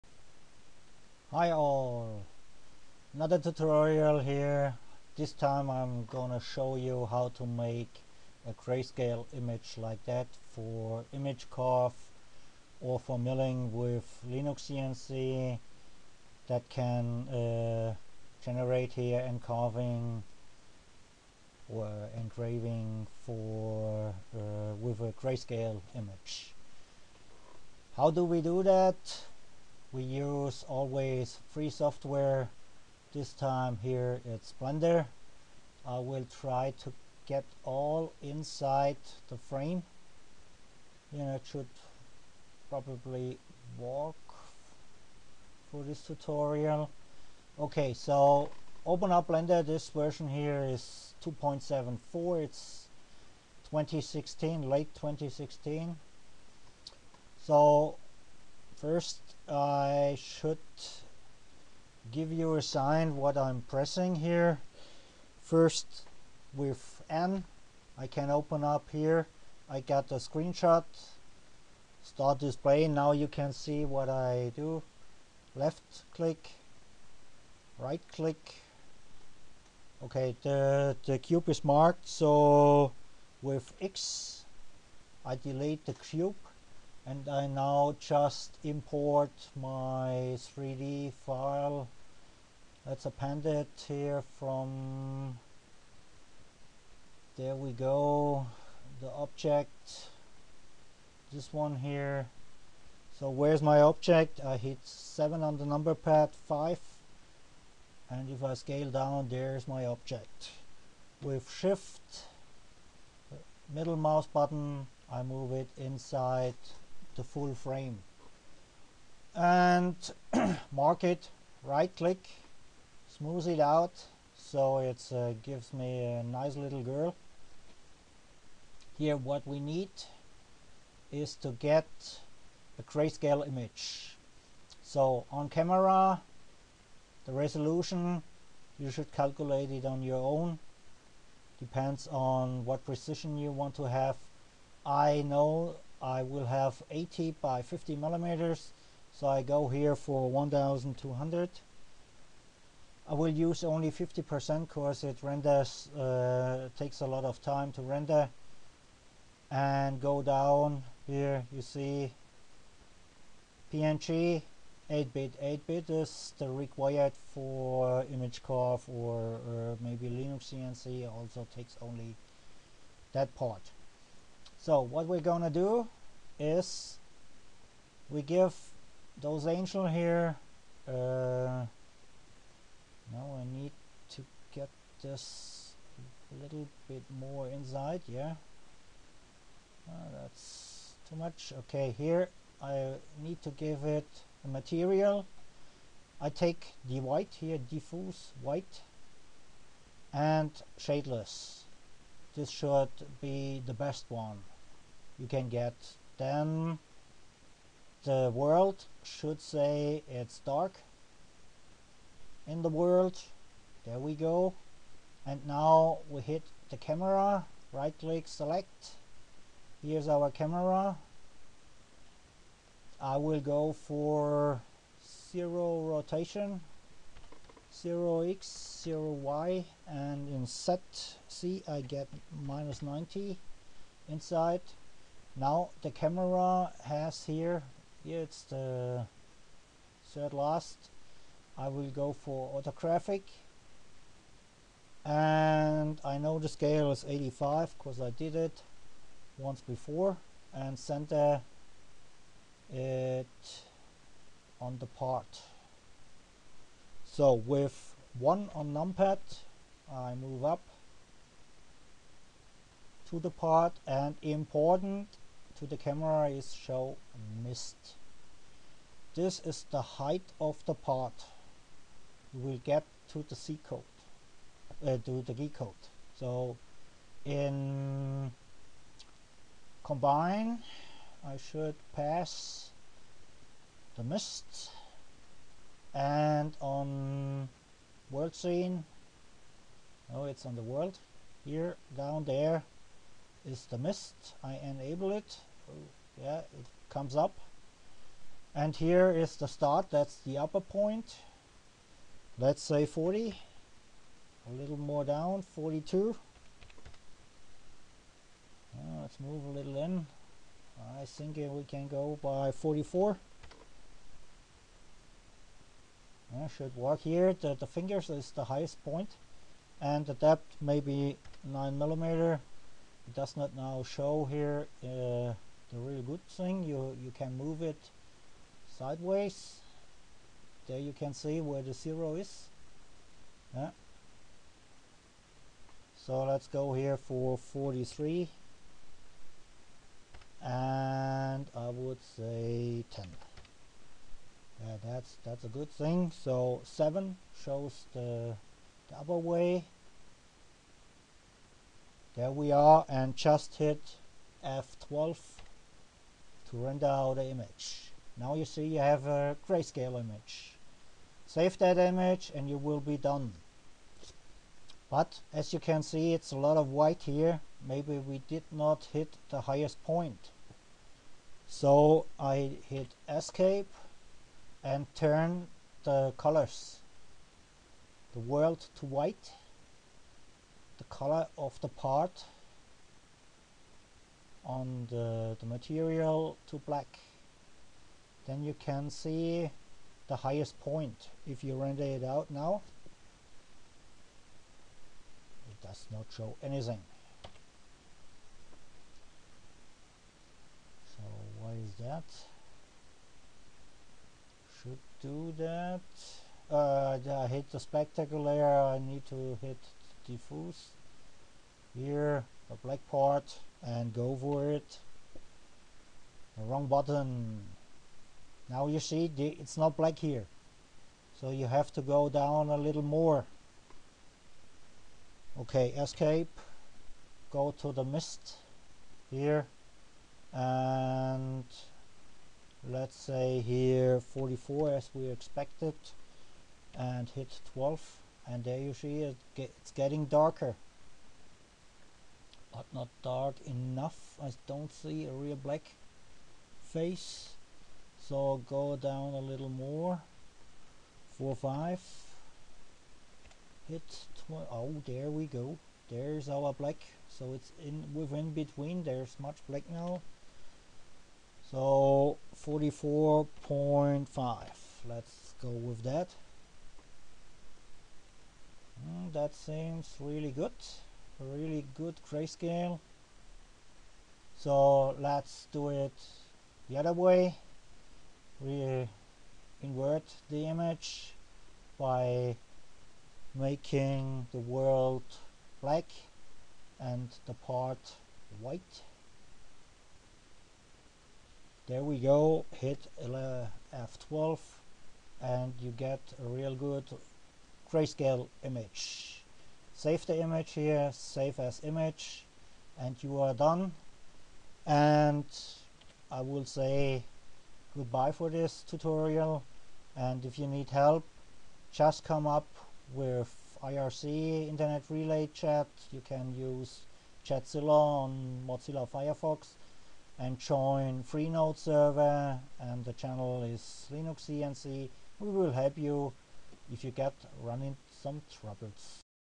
Blender 3D Grayscale Hightmap
The Video is on howto generate a Height map